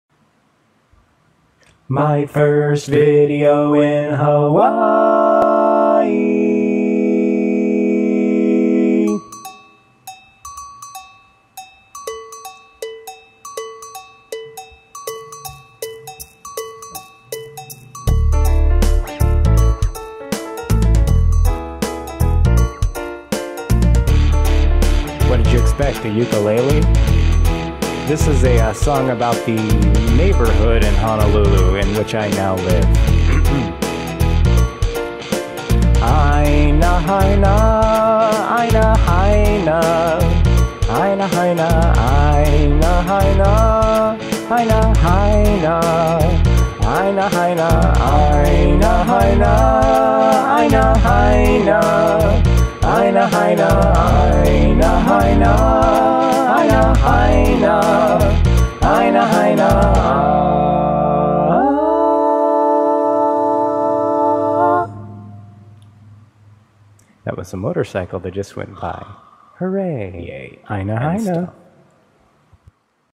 Aina Haina (My first Hawaiian video)
So, I'm in Hawaii about to start my PhD at UH Manoa, and while I'm still waiting to hear back from possible part time jobs and health records, I decided to make a video/musical tribute to my new neighborhood.